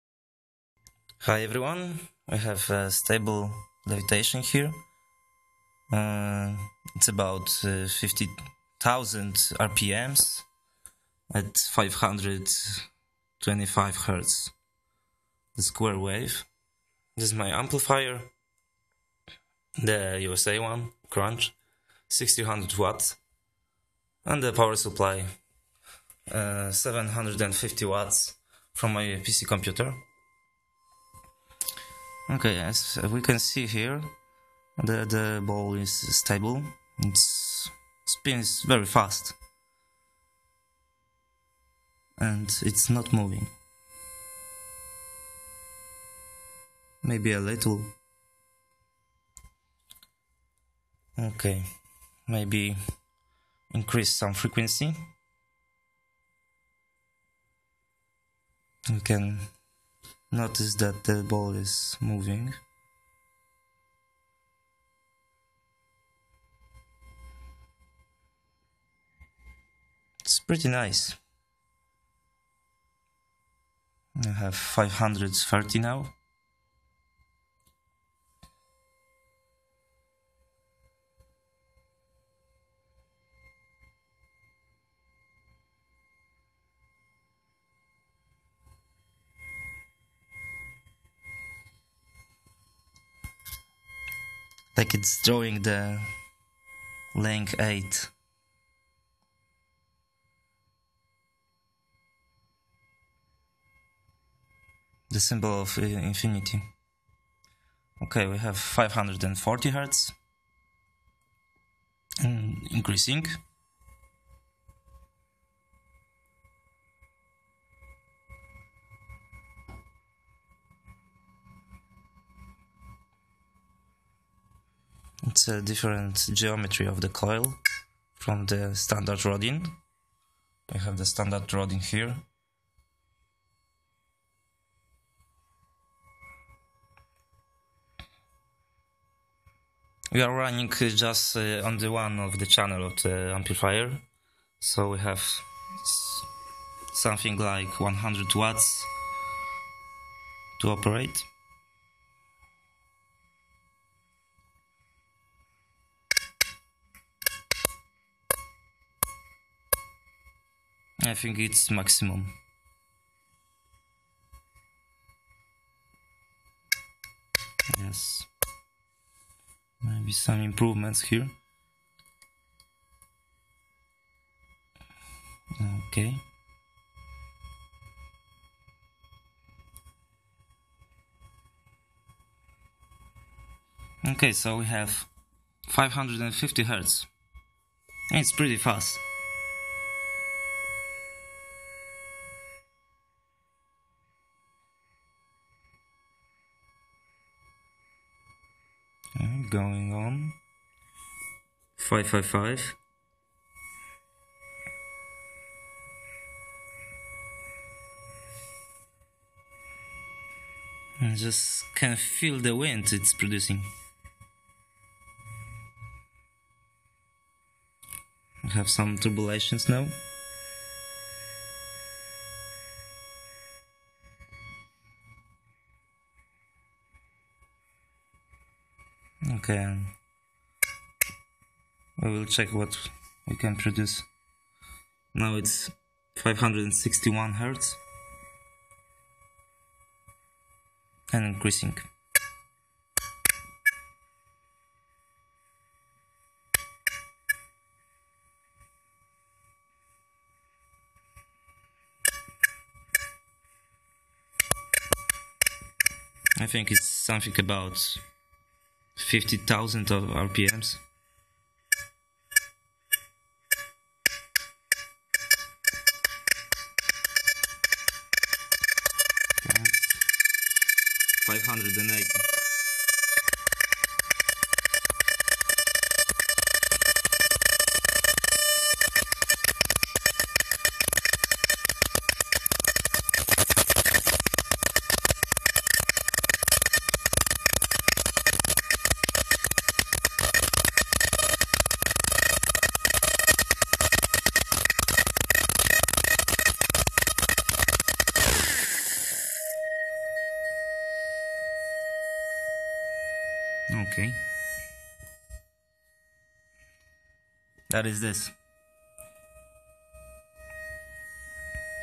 STABLE LEVITATION via RODIN coil
Stuff:
-neodymium ball 13mm
-Rodin interference coil, torus outer diameter 134,59mm, inner diameter 42mm, wire diamater 0,55mm
-DDS signal generator 0-5Mhz, stability ± 1 × 10-6, accuracy ± 5 × 10-6
-amplifier Crounch ONE 600W 1 Ohm stable, here used just one channel at minimum amplification
-power supply for amplifier - PC PSU Seasonic 750W